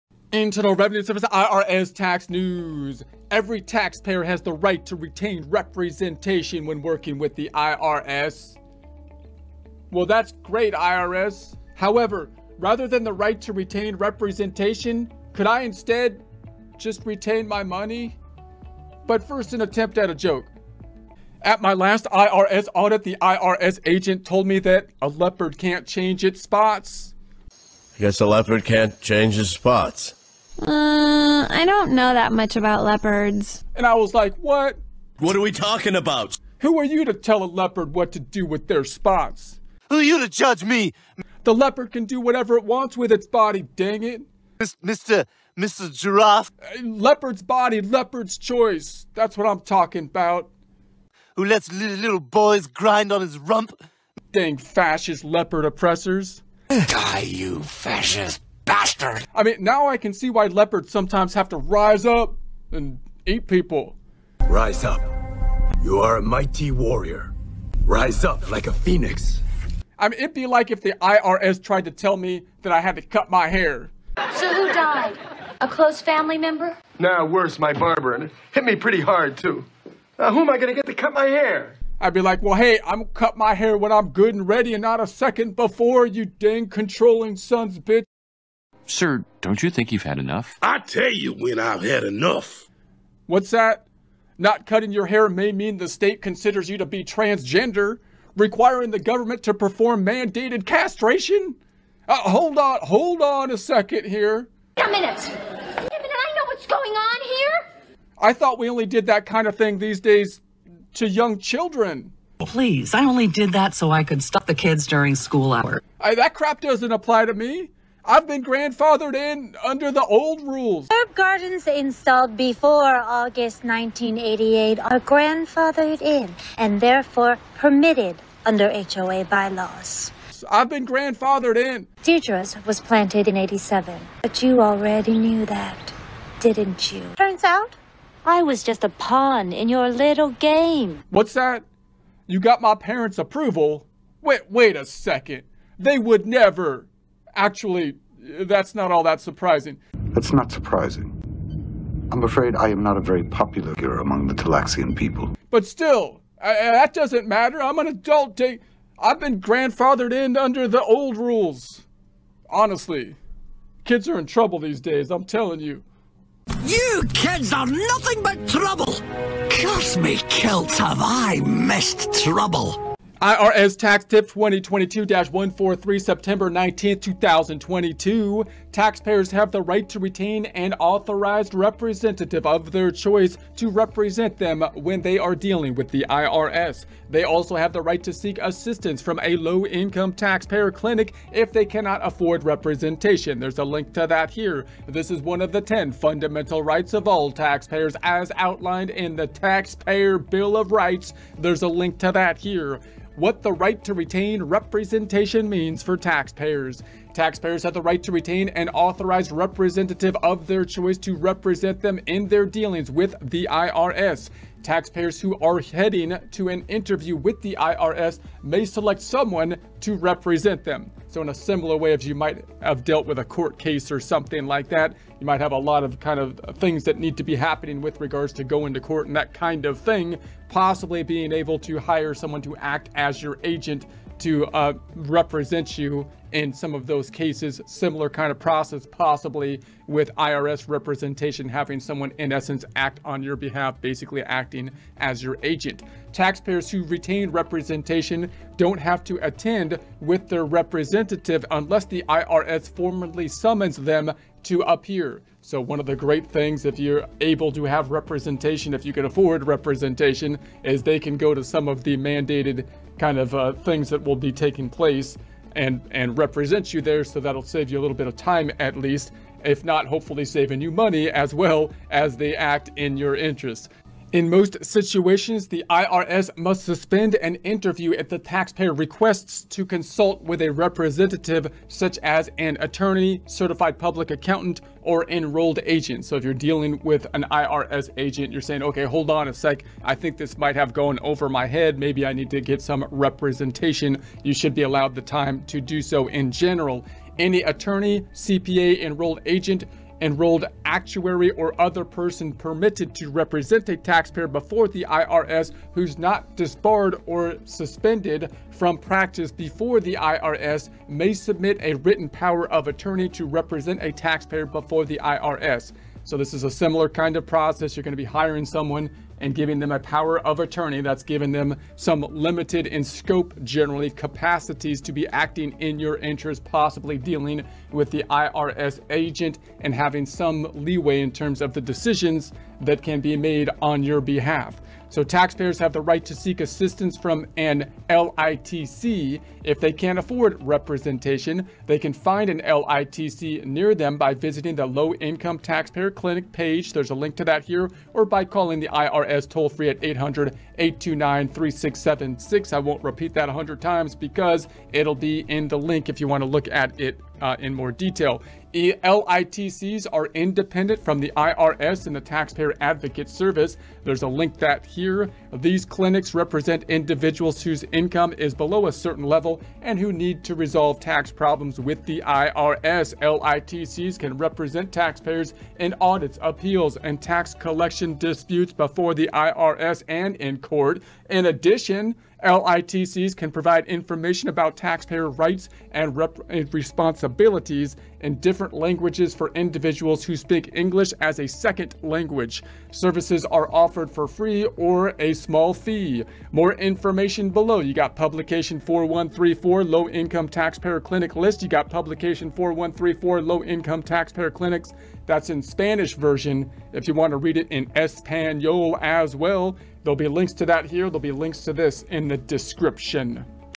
Every taxpayer has the right to retain representation when working with the IRS
Taxpayers have the right to retain an authorized representative of their choice to represent them when they are dealing with the IRS. They also have the right to seek assistance from a Low Income Taxpayer Clinic if they cannot afford representation. This is one of the ten fundamental rights of all taxpayers as outlined in the Taxpayer Bill of Rights.

What the right to retain representation means for taxpayers:
Taxpayers have the right to retain an authorized representative of their choice to represent them in their dealings with the IRS.
Taxpayers who are heading to an interview with the IRS may select someone to represent them.
Taxpayers who retain representation don't have to attend with their representative unless the IRS formally summons them to appear.
In most situations, the IRS must suspend an interview if the taxpayer requests to consult with a representative, such as an attorney, certified public accountant or enrolled agent.
Any attorney, CPA, enrolled agent, enrolled actuary or other person permitted to represent a taxpayer before the IRS, who's not disbarred or suspended from practice before the IRS, may submit a written power of attorney to represent a taxpayer before the IRS.
Taxpayers have the right to seek assistance from an LITC if they can't afford representation. They can find a LITC near them by visiting the Low Income Taxpayer Clinics page or by calling the IRS toll-free at .
LITCs are independent from the IRS and the Taxpayer Advocate Service These clinics represent individuals whose income is below a certain level and who need to resolve tax problems with the IRS. LITCs can represent taxpayers in audits, appeals and tax collection disputes before the IRS and in court. In addition, LITCs can provide information about taxpayer rights and responsibilities in different languages for individuals who speak English as a second language. Services are offered for free or a small fee.

Publication 4134, Low Income Taxpayer Clinic ListPDF
Publication 4134, Low Income Taxpayer Clinics, Spanish version